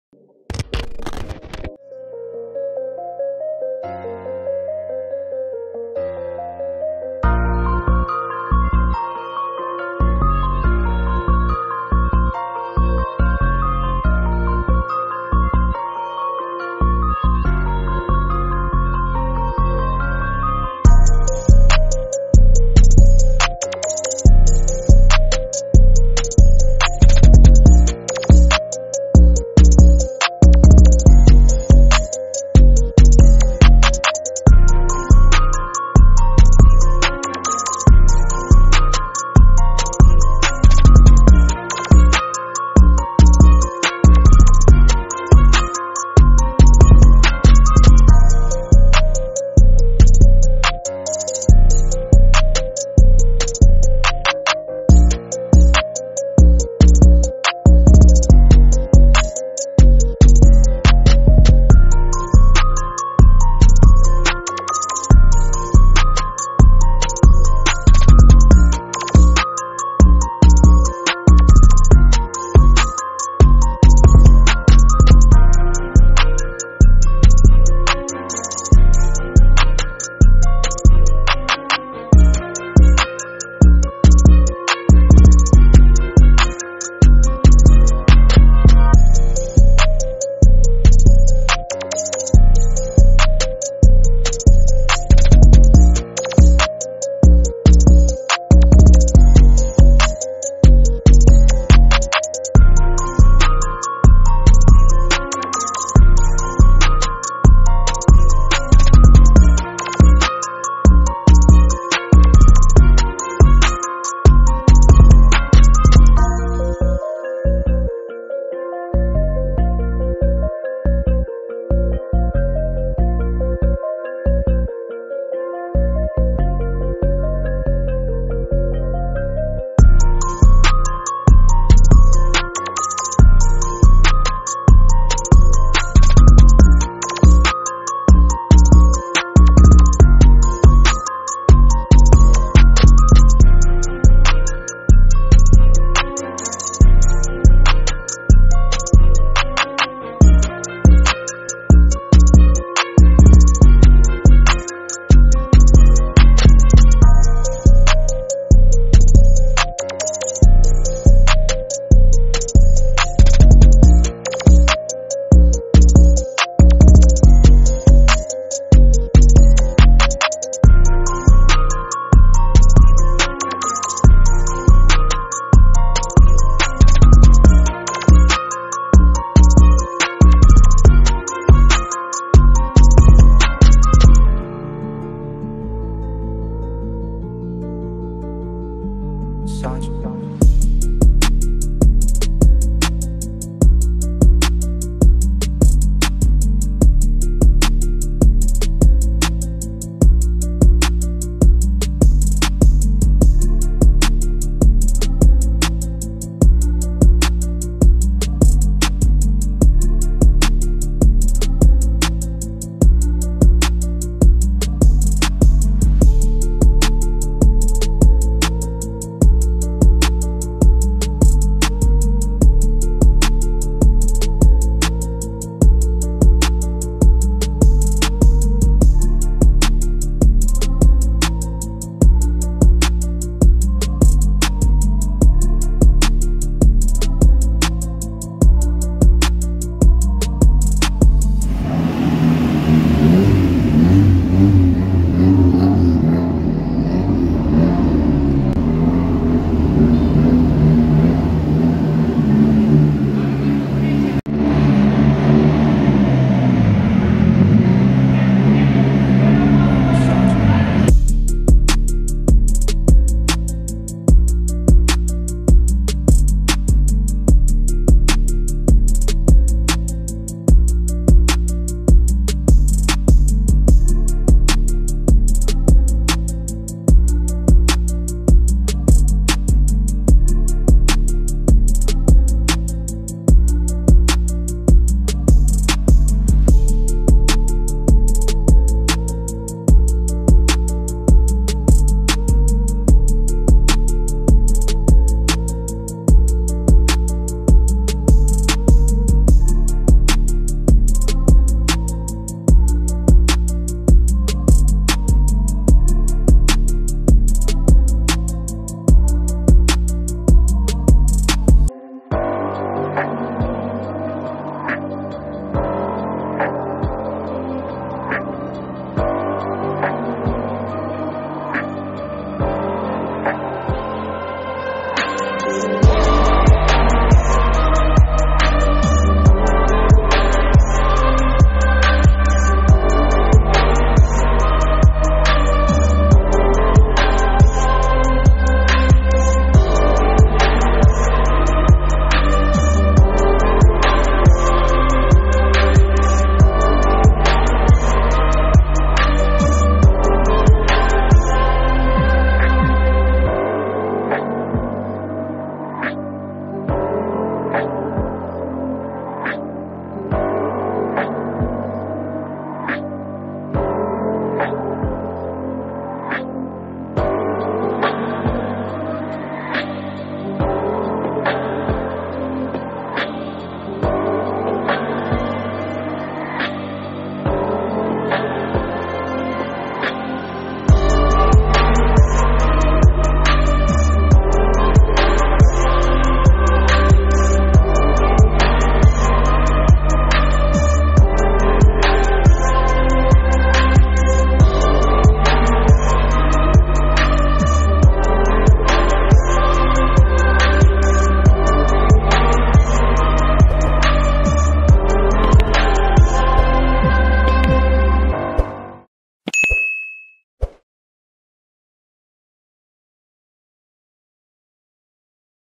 10 anos de Só Fiat Uno
Em comemoração aos 10 anos da equipe Só Fiat Uno, a galera se reuniu num dos espaços mais completos de sp, no Garagem 55 na móoca, local ideal para curtir com a família.
O evento contou com diversos projetos diferenciados, originais e modificados, antigos e atuais. Com a presença de clubes, o encontro proporcionou uma variedade de modelos exclusivos cada um com seus detalhes e suas histórias.
Um evento top pra tirar os projetos da garagem e encontrar os amigos pra trocar aquela idéia.
Obs: Um Agradecimento especial para a galera do 147 Adaptações

Se você não pôde estar presente, da um play e da um confere nesse super evento!

👇Não perca, confira o evento More Respect, um rolê realizado no bairro Itaim Paulista zona leste de SP👇